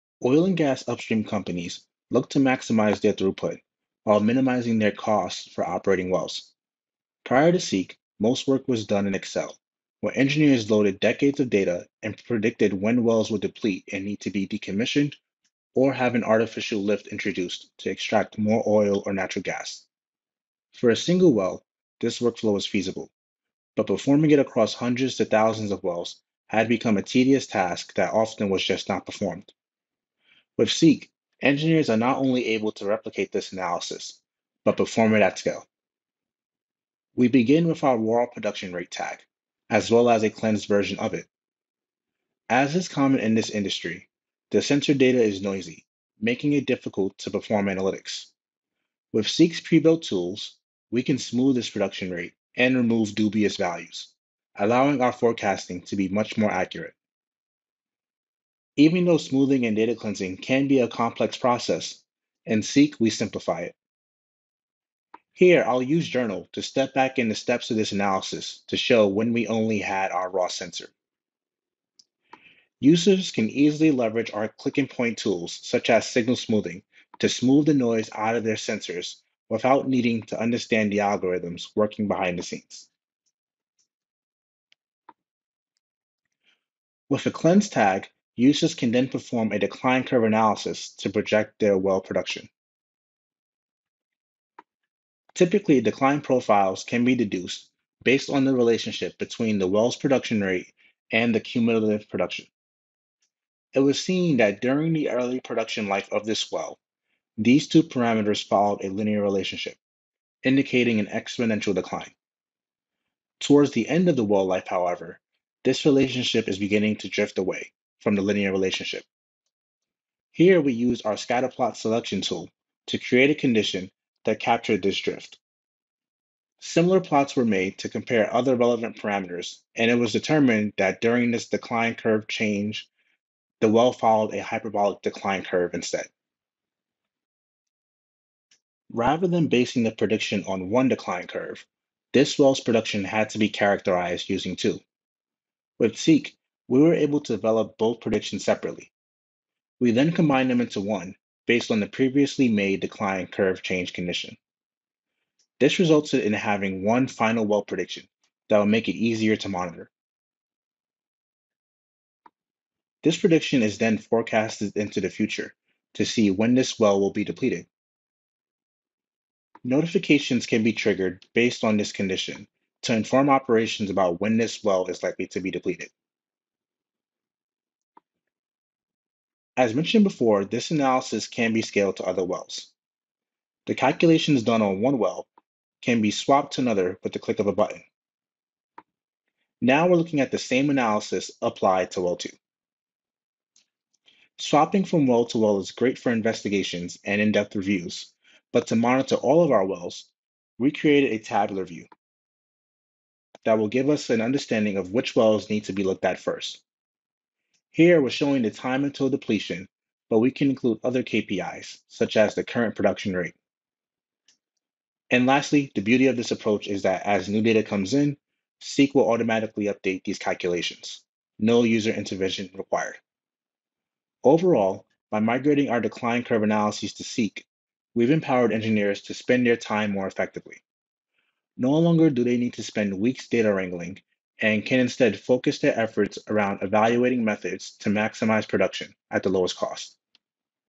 Seeq | Use Case: Well Life Depletion Forecast
With increasing operational costs, exploration and production oilandgas companies are benefitting from improved well surveillance. Well-life forecasts, often called decline curve analyses, can provide crucial information for maximizing throughput and minimizing costs. Traditionally done in Excel, these analyses were difficult to create and maintain, meaning only the top few wells were monitored regularly. With Seeq, engineers can develop intuitive predictive models that update as new data becomes available and easily scale these analyses to other wells, enabling regular monitoring over the entire fleet.